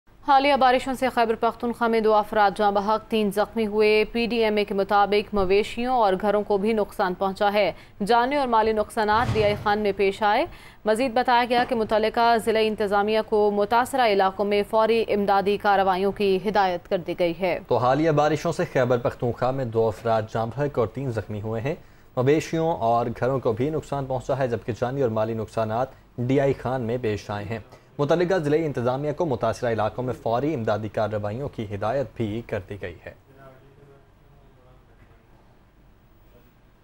Exclusive! KPK Me Tufani Barishon Se Tabahi
خیبر پختونخواہ میں طوفانی بارشوں سے تباہی
کتنے افراد جاں بحق؟الرٹ جاری